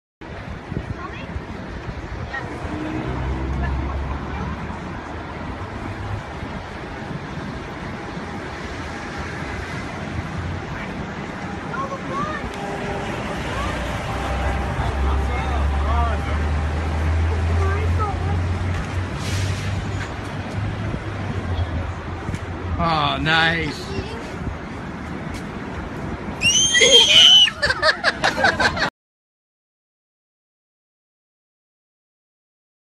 LeBron James Emotional Moment With A Young Fan Leaving Him Speechless!
LeBron James Emotional Moment With A Young Fan Leaving Him Speechless! The kid's Instagram is LeBron James saw the young fan waiting for him and the fan kindly asked James for a picture and was left speechless.